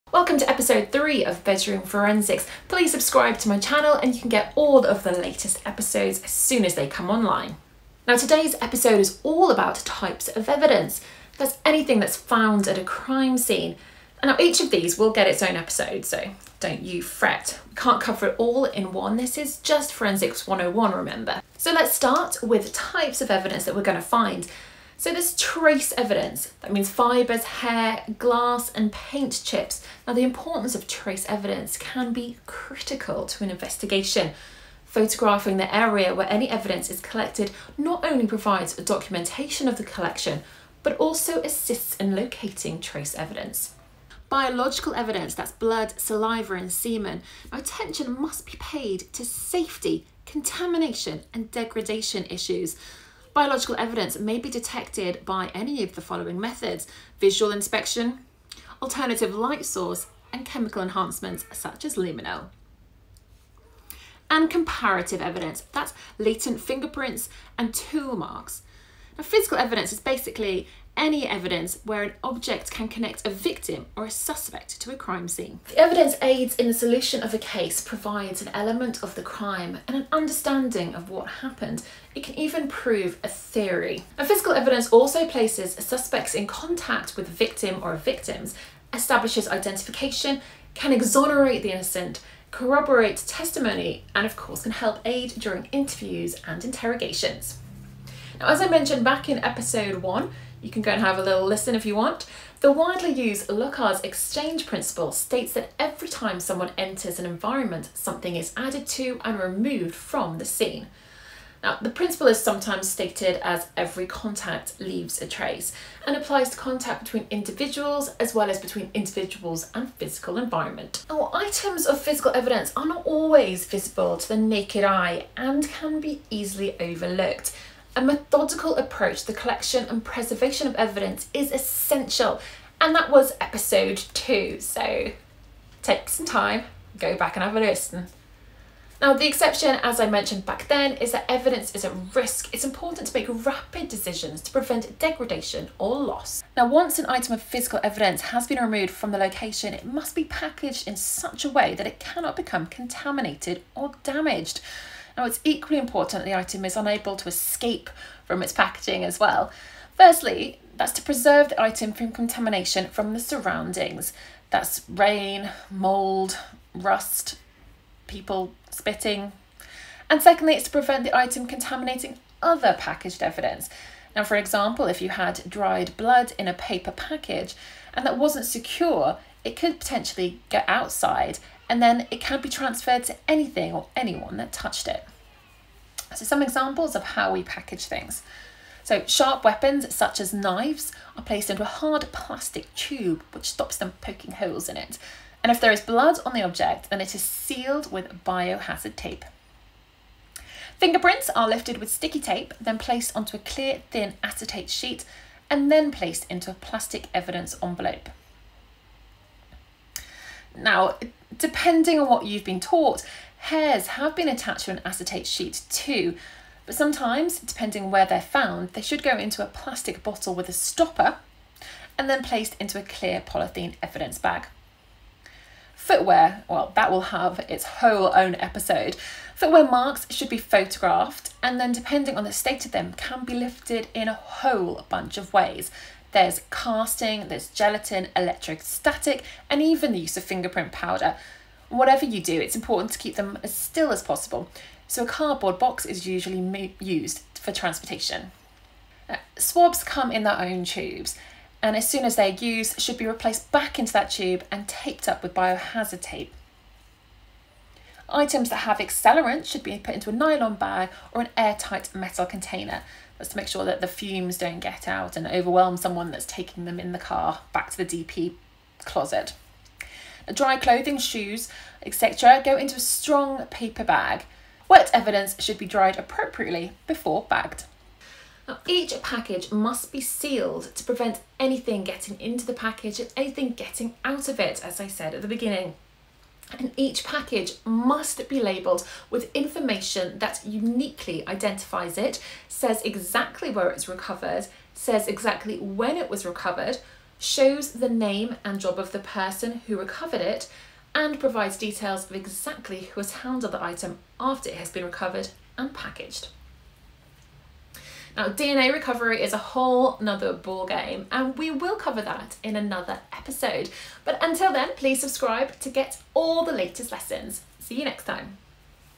All about that blood, semen, and evidence... Episode three, Bedroom Forensics
let's catch those criminals and let all the innocent people go!

Alex Iszatt looks at physical evidence and whether it will either prove or disprove statements of what may or may not have happened.

You can buy any of the packaging from a range of forensic outlets - including Latentforensicsdotcom, where I snaffled the pic of the knife tube - thanks guys.